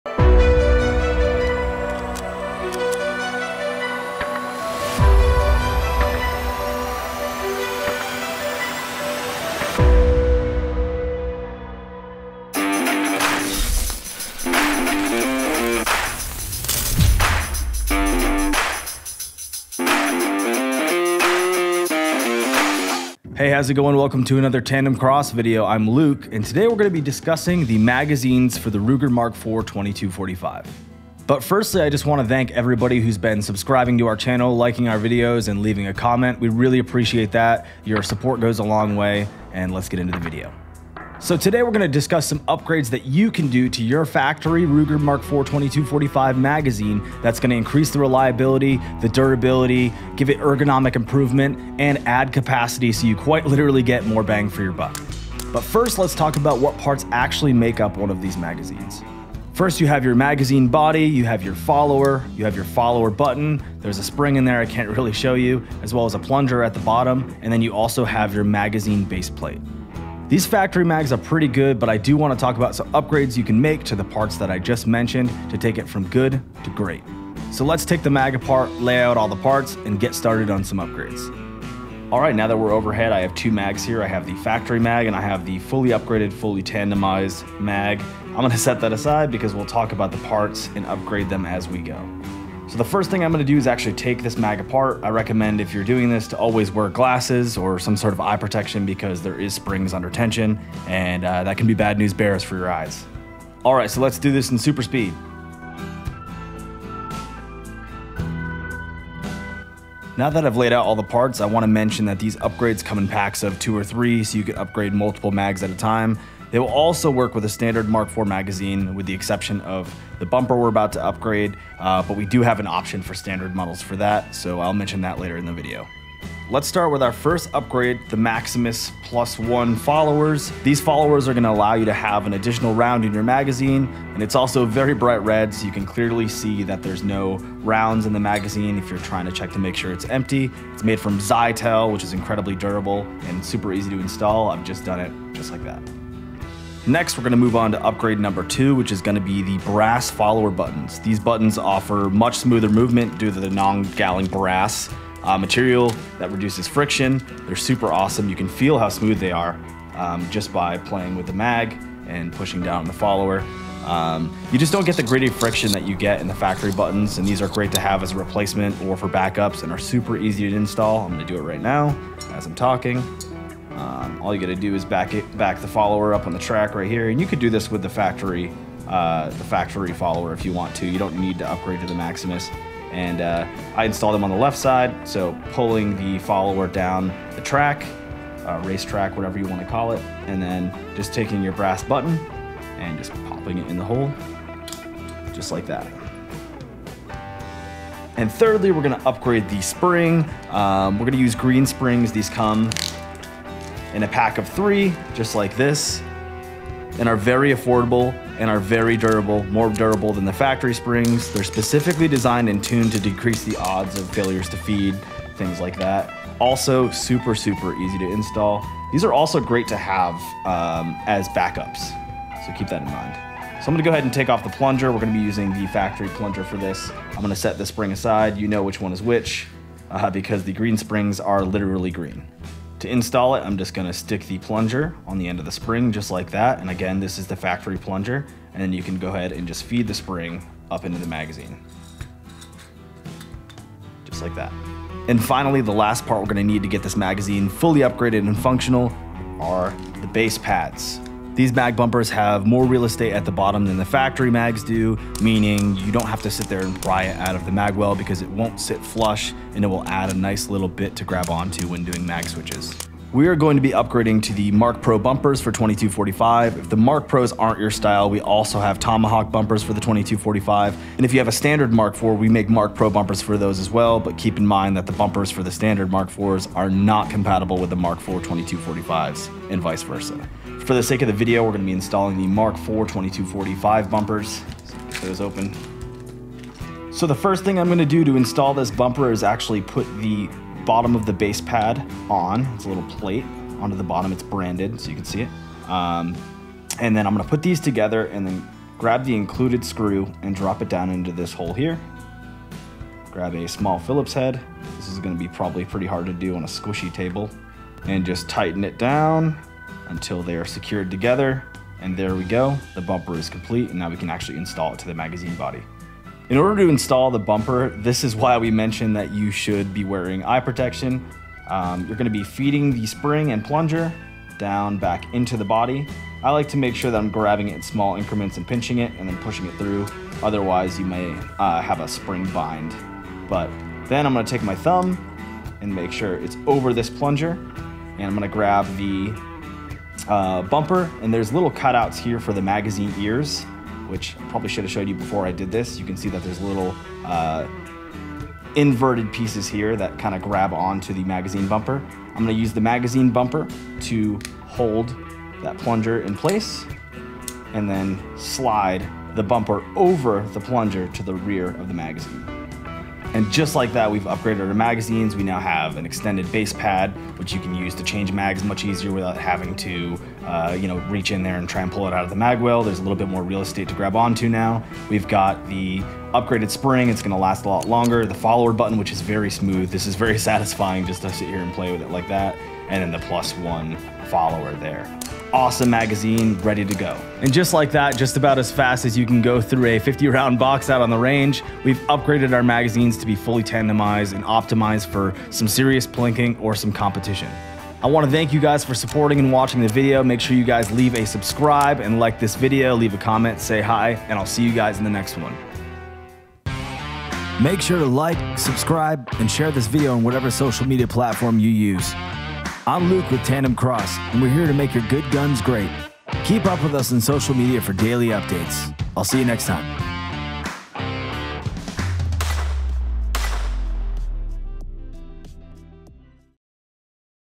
Upgrading Your Ruger Mark IV & 22/45 Magazines!
Join Luke as he discusses the benefits of upgrading a factory Ruger Mark IV 22/45 magazine from good to GREAT with Tandemkross!

Factory magazines are good, but these upgrades will extend the life of your magazines, increase round count, add ergonomics for mag changes, reduce failures to feed, and make operation much more smooth and satisfying.